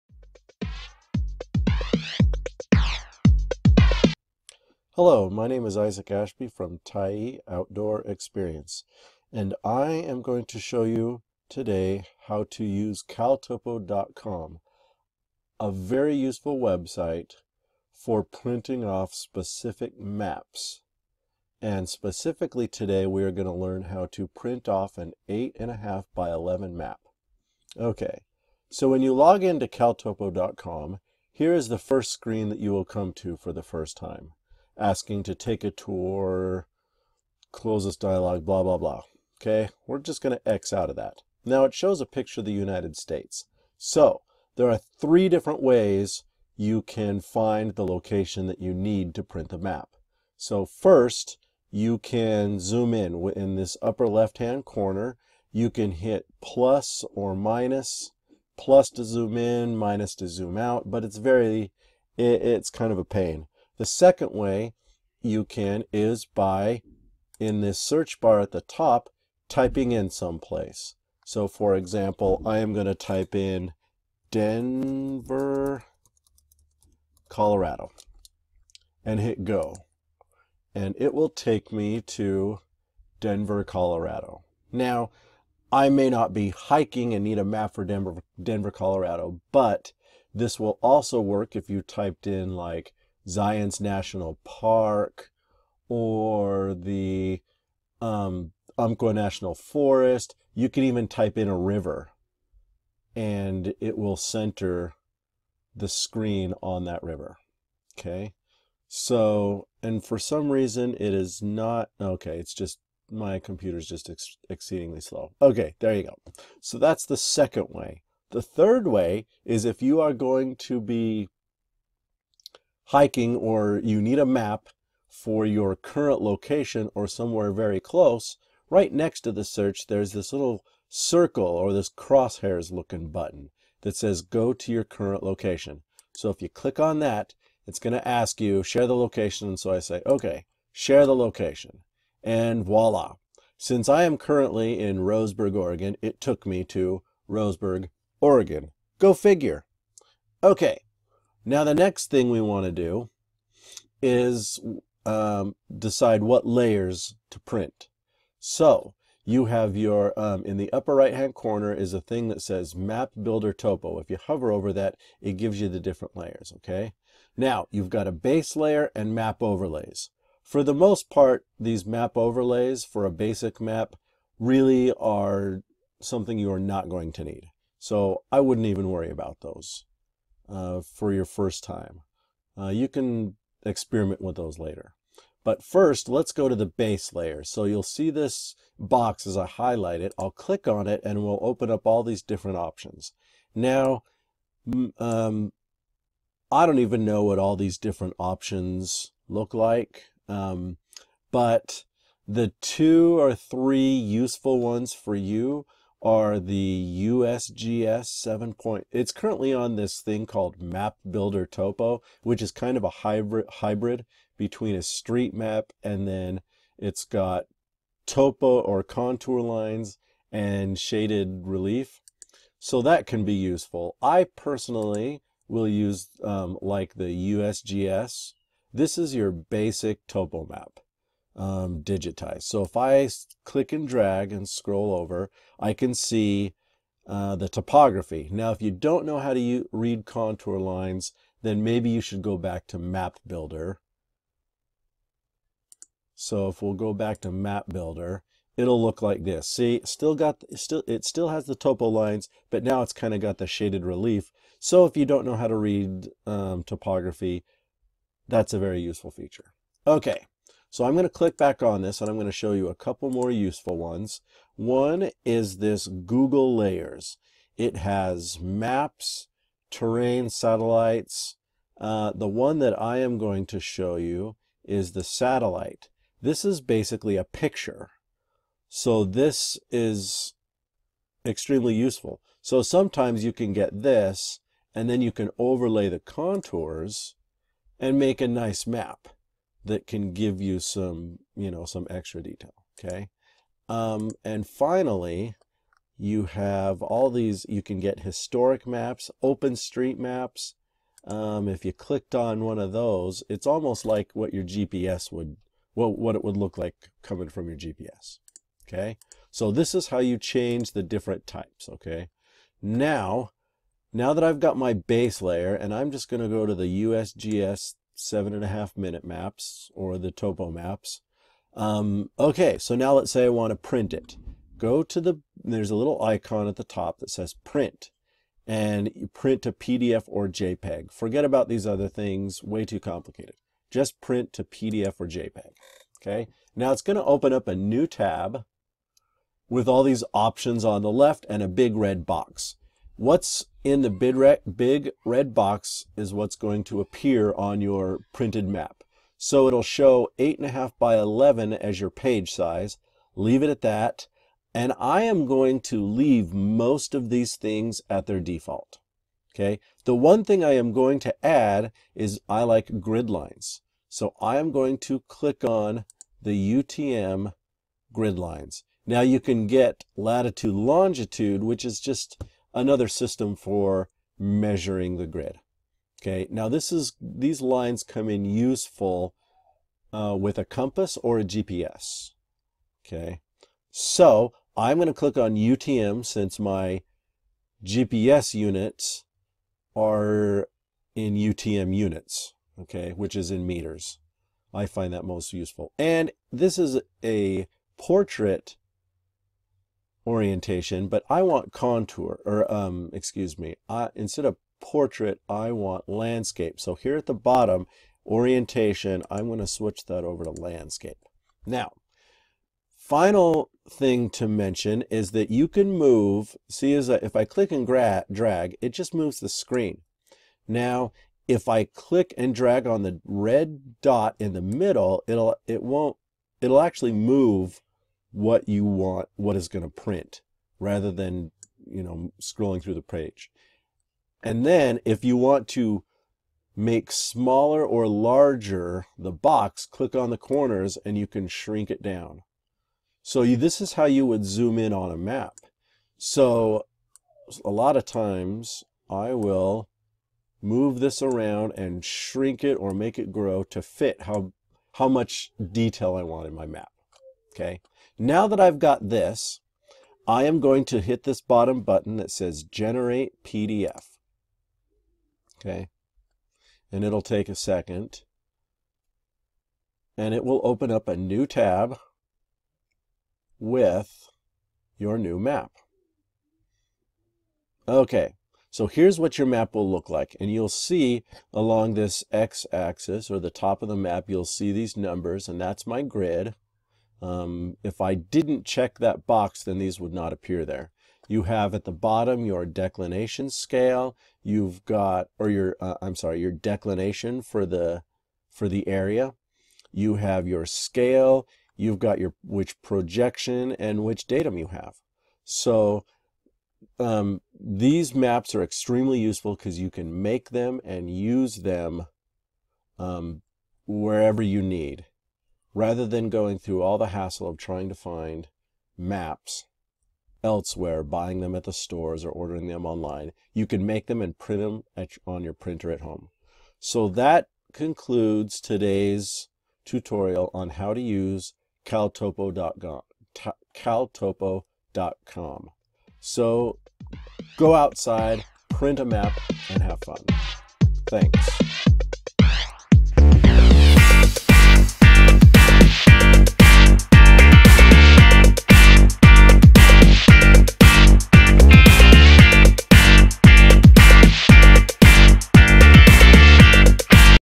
How to Quickly Print Free Maps with Caltopo - Navigation
When things go bad, your GPS will only be good as a paperweight.  Take a map!

With Caltopo.com you can quickly and easily print maps for all your adventures. High quality topographical maps (TOPO maps) for the U.S., Canada, & New Zealand available.  General streetmaps are available worldwide.

These are great for teaching kids basic navigation too!  They are not the big, expensive maps and you can print a lot of them easily.

A shoutout to Matt Jacobs for this amazing resources!